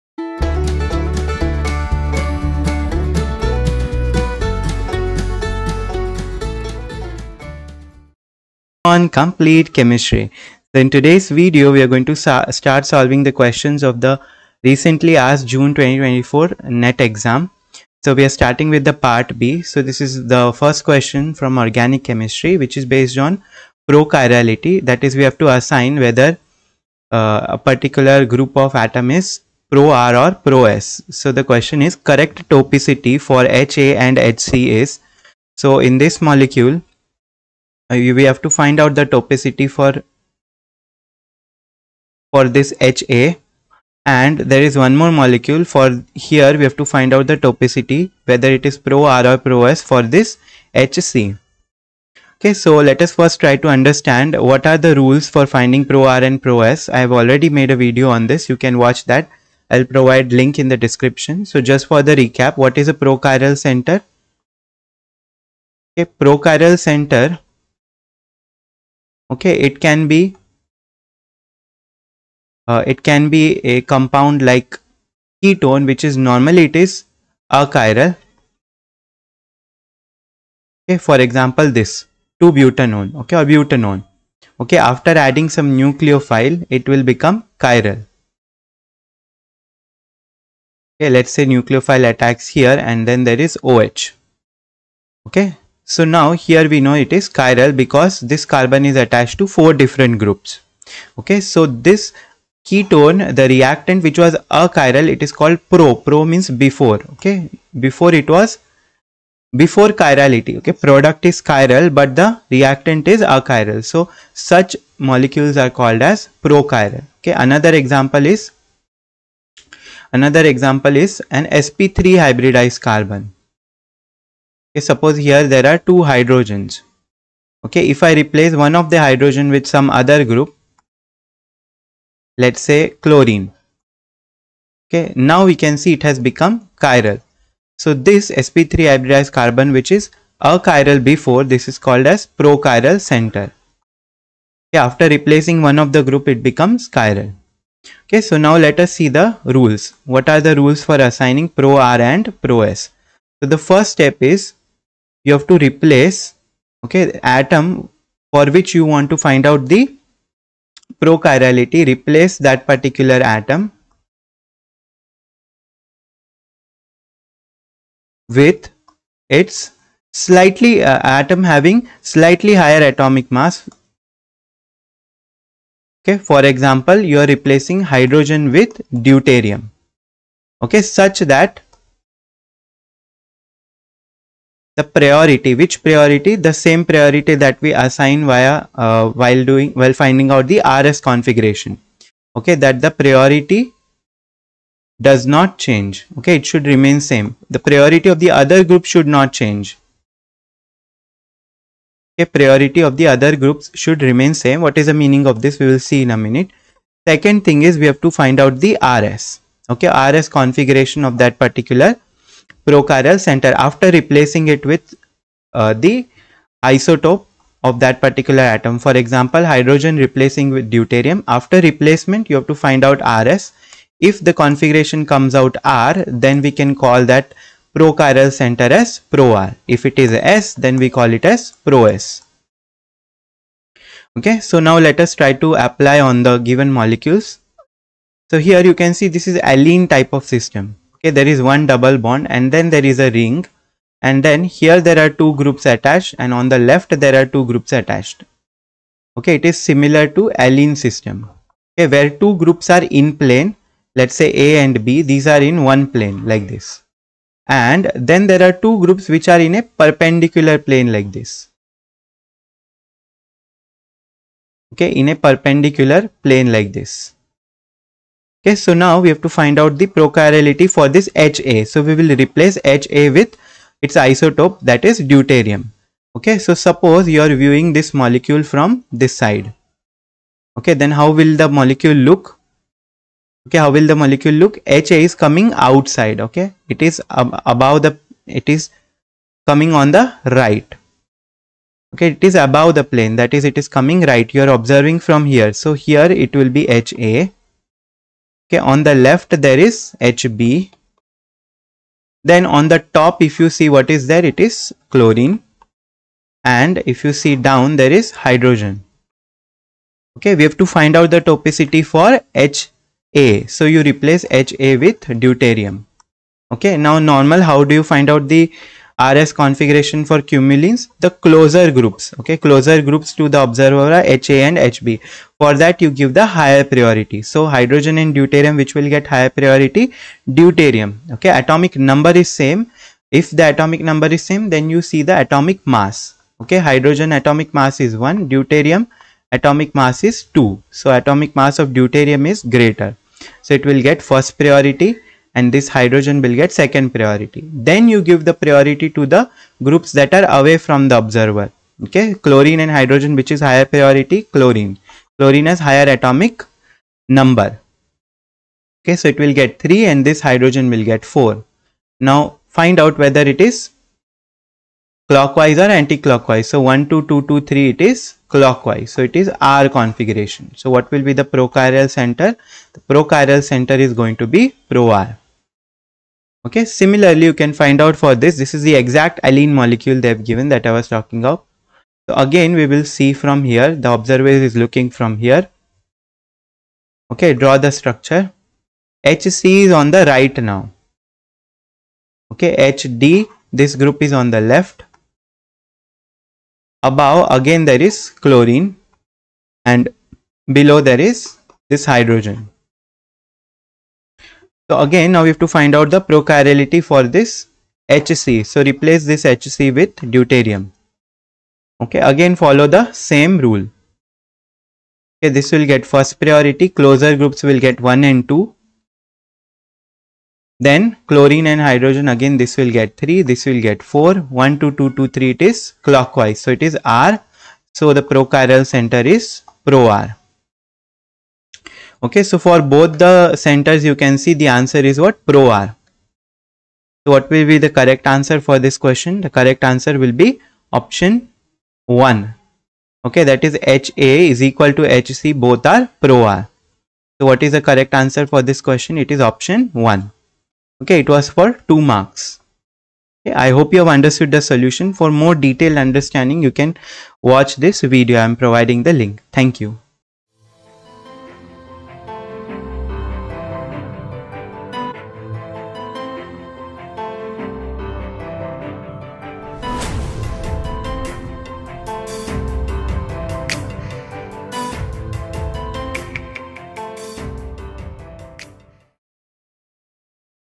Pro-R and Pro-S | Prochiral Center | Topicity | CSIR NET June 2024 Chemical Science (part B)
Hello Everyone!!!

In today's video, we are going to solve a question based on prochiral centre or prochirality where we have to find pro-R or pro-S, that was asked in part B of CSIR NET June 2024 Chemical Science paper.